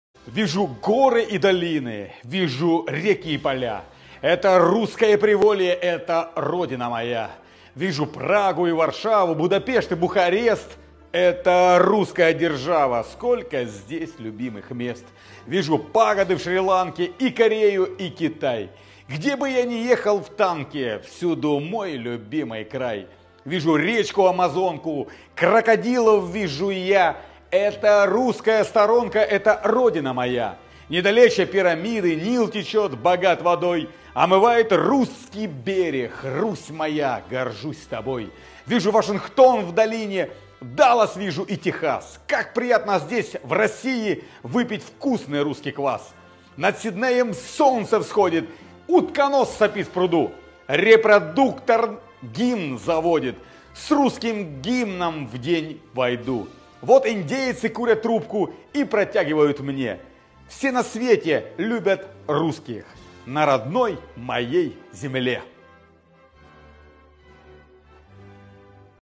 Бред путиноида // Кирилл Стремоусов
Кого ждет Гаага, кого Кащенко...
«Вижу Прагу и Варшаву, Будапешт и Бухарест —это русская держава, сколько здесь любимых мест», — заместитель главы ВГА Херсонской области Кирилл Стремоусов объединил весь мир под эгидой России 🤯